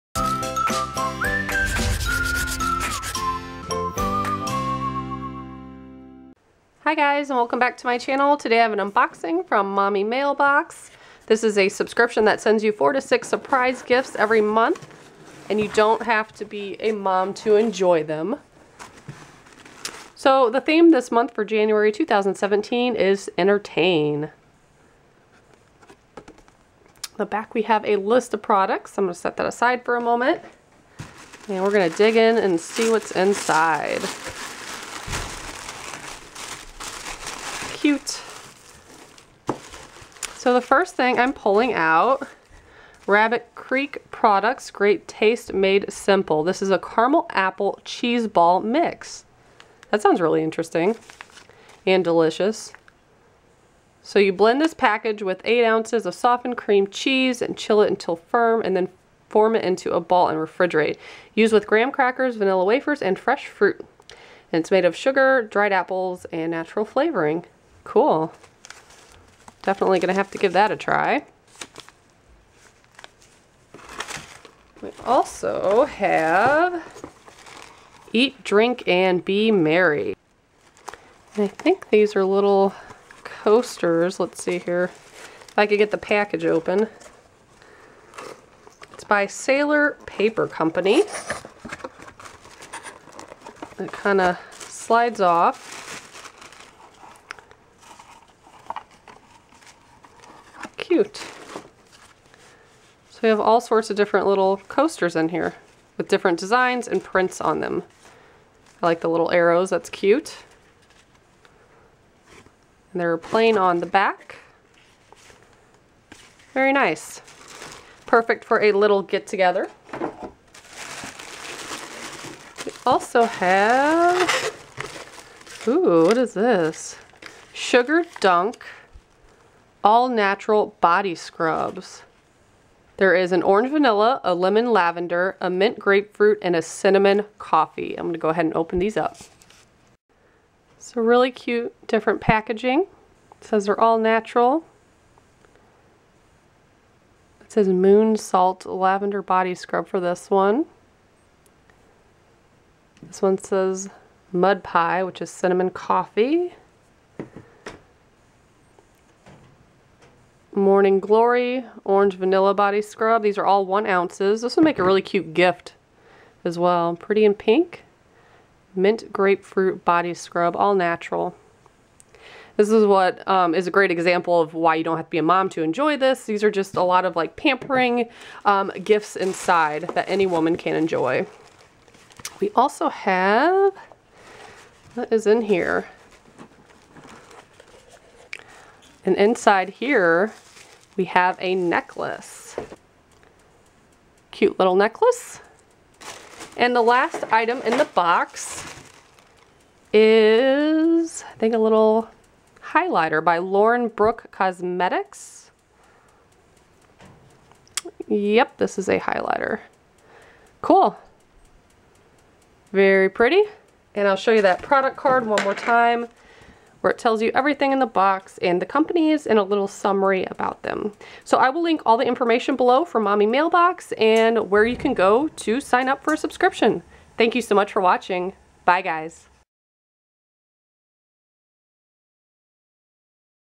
Mommy Mailbox Subscription Unboxing - January 2017!
Each box contains 4-6 surprise gifts.

Inside you will find items like chocolate, gourmet food treats, jewelry, makeup and beauty items, fashion accessories, stationery and other lifestyle type of products.

*SHIPPING RATES APPLY TO ORDERS OUTSIDE THE UNITED STATES

This is a box that all women love--you don't have to be a mommy!

The January 2017 Entertain box contained the following items:
• Coasters by Sailor Paper Company
• Highlighter by Lauren Brooke Cosmetiques
• Body Scrubs by Sugar Dunk
• Cheeseball Mix by Rabbit Creek Gourmet
• Necklace by Henlie